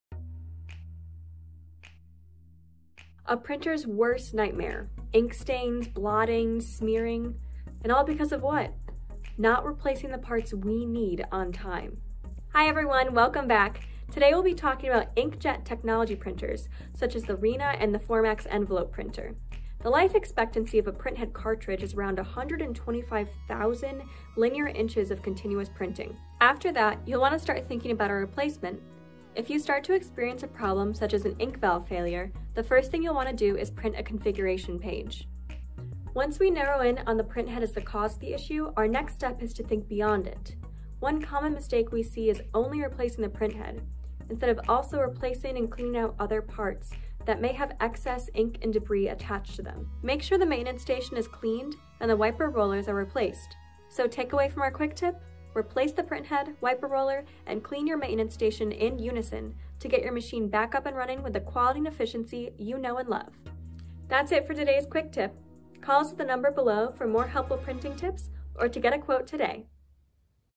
QuickTip: Envelope Printhead
If your printhead starts to go, don't forgot to also change the wiper rollers and clean the maintenance station at the same time! P.S.- This way you won't have any excess ink and debris on your machine.
Transcript:
A printer’s worst nightmare: Ink stains, blotting, and smearing, and all because of what? Not replacing all the parts, we need on time.

Today, we’ll be talking about InkJet Technology printers such as the RENA and Formax Envelope Printers.

Our life expectancy for a printhead cartridge is about 125,000 linear inches of continuous printing. After that, we’ll want to start to think about a replacement. If you start to experience a problem such as an ink valve failure, the first thing you’ll want to do is print a configuration page. Once we narrow in on the printhead as the cause of our issue, our next step is to think beyond the printhead. A common mistake we see is only replacing the printhead without also replacing and cleaning our other parts that may have excess ink and debris attached to them.

Make sure the maintenance station is cleaned and the wiper rollers are replaced. So takeaway from our QuickTip? Replace the printhead, wiper roller, and clean your maintenance station in unison to get your machine back and running with the quality and efficiency you know and love!